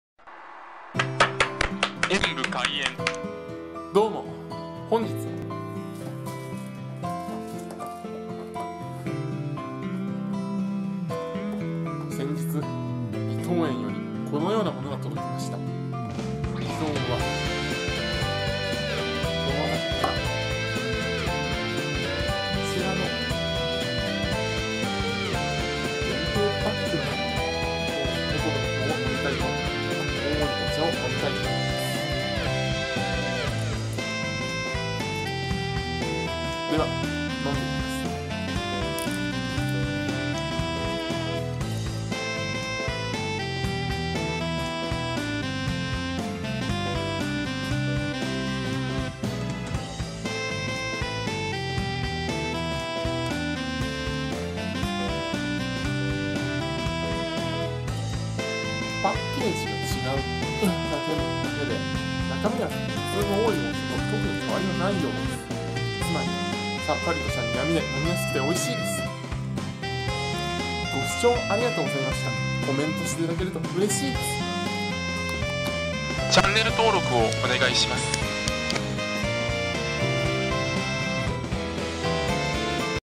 伊藤園 紅葉パッケージのおーいお茶を飲んでみた
伊藤園　紅葉パッケージのおーいお茶を飲んでみました。
秋が今から待ち遠しいです！！